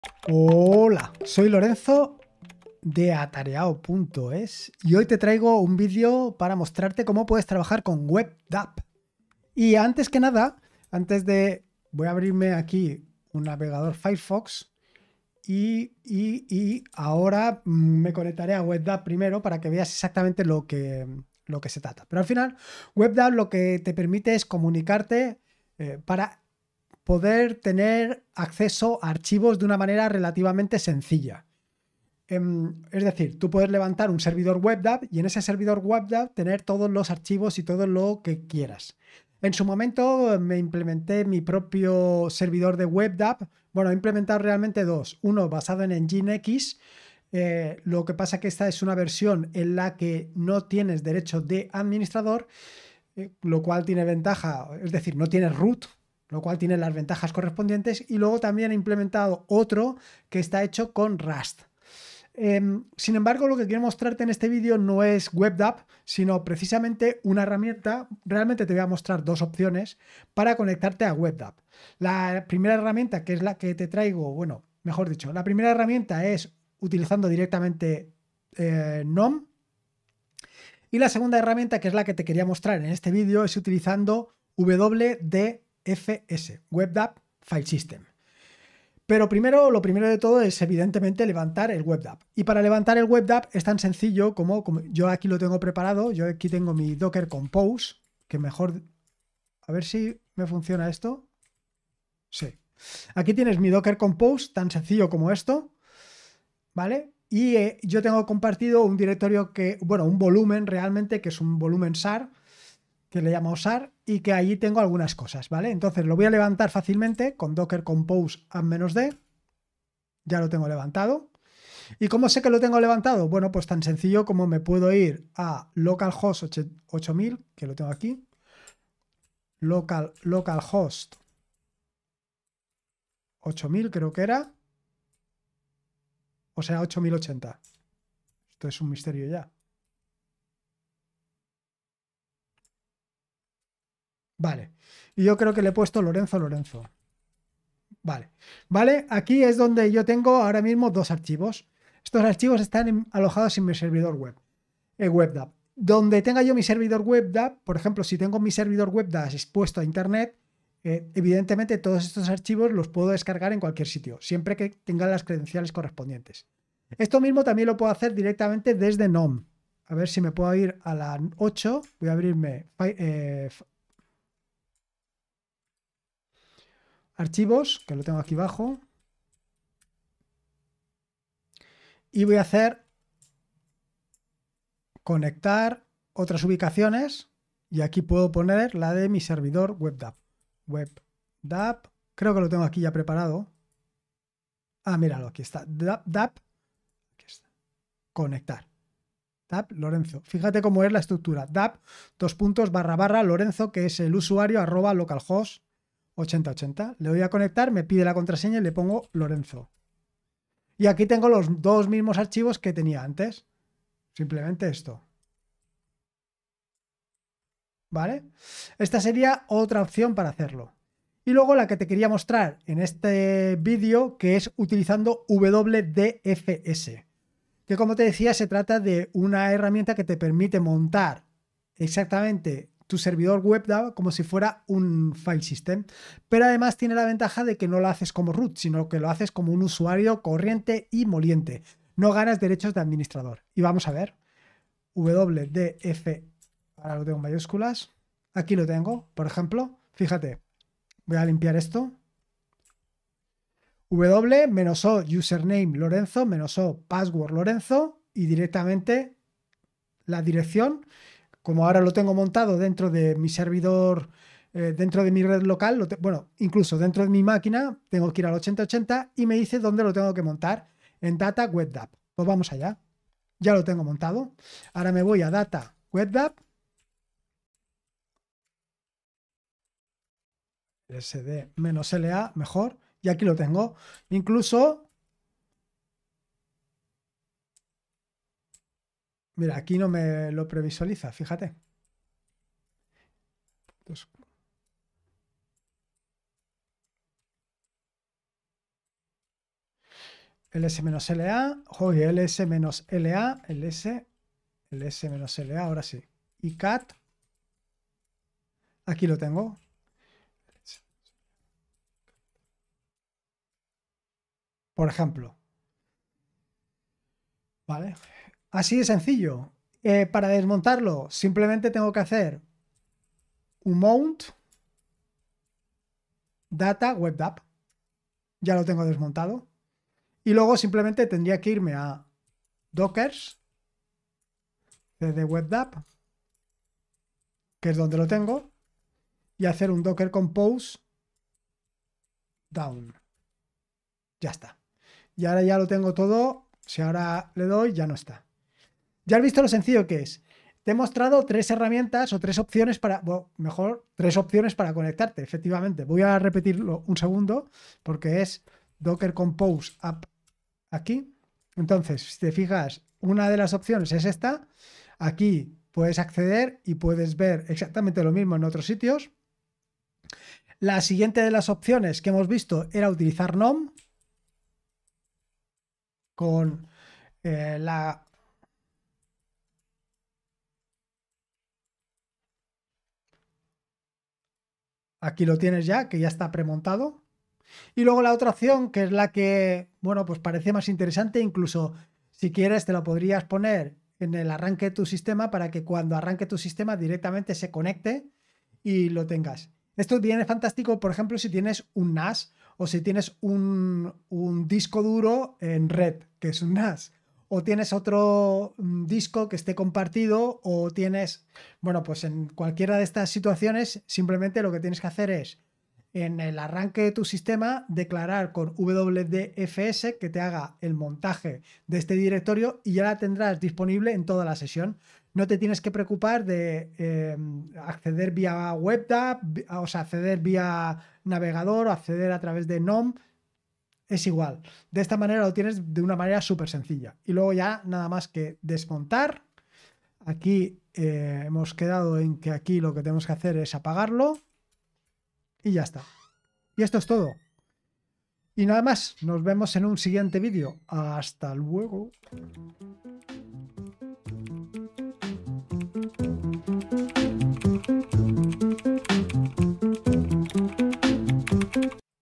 Utilizar WebDav en Linux
En este vídeo te muestro como puedes utilizar webdav en linux con diferentes opciones, desde el navegador web, pasando por gnome, en concreto files para terminar con una interesante herramienta como es wdfs un filesystem para webdav.
Además te muestro como puedes levantar de forma muy sencilla tu servidor webdav utilizando Docker.